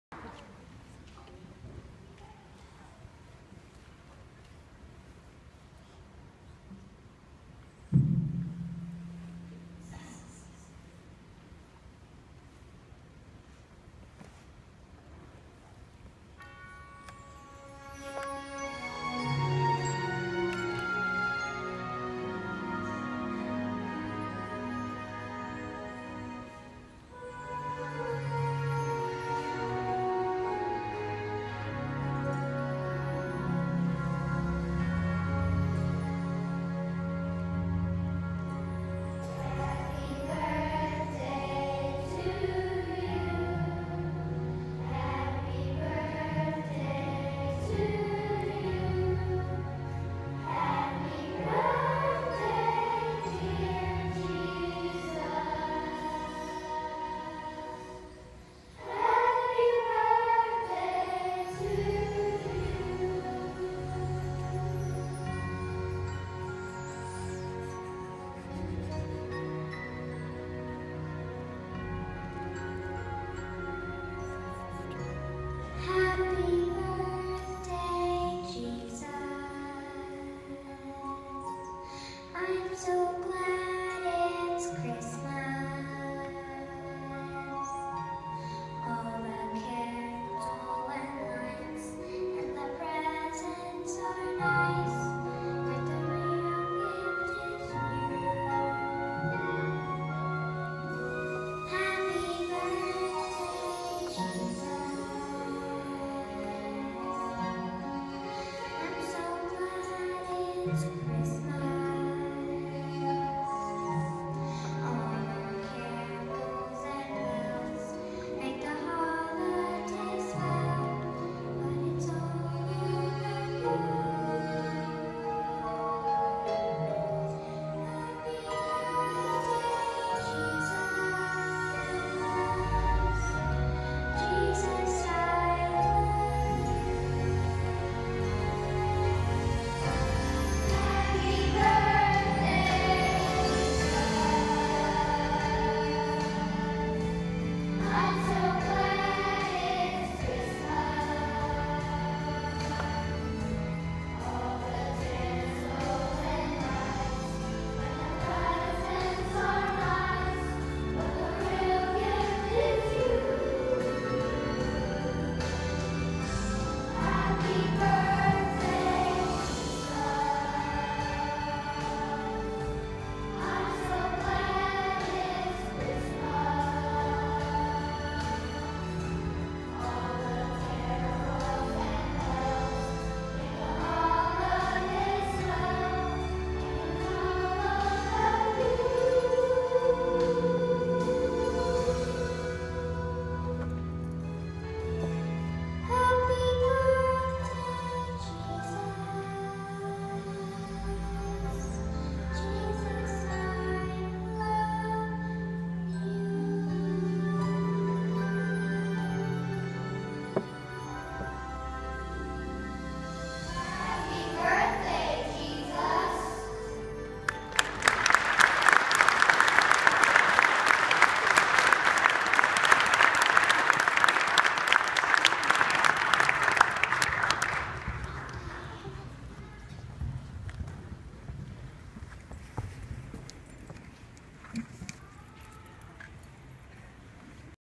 Brooklyn's solo
My niece singing her solo at a Christmas program